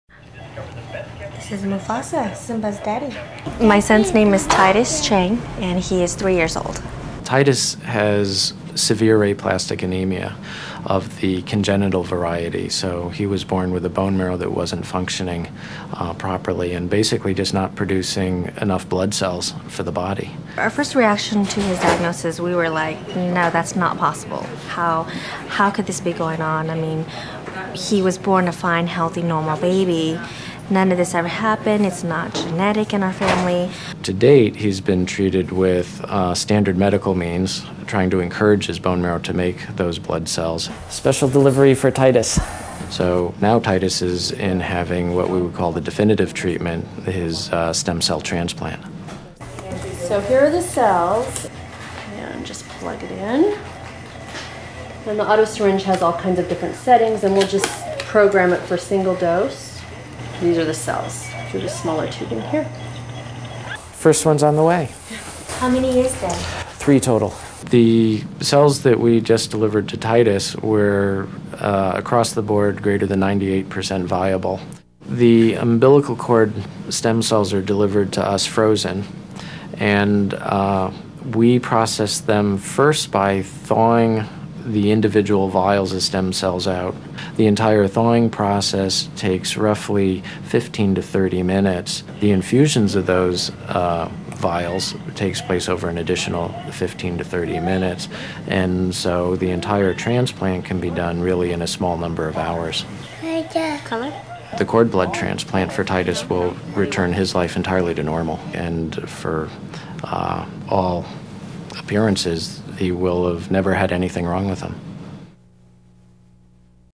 Cord Blood Stem Cell Transfusion Saves 3-Year-Old Child
Diagnosed at birth with aplastic anemia, Titus Chang was saved by a cord blood transplant from his newborn brother.  and request a free info kit.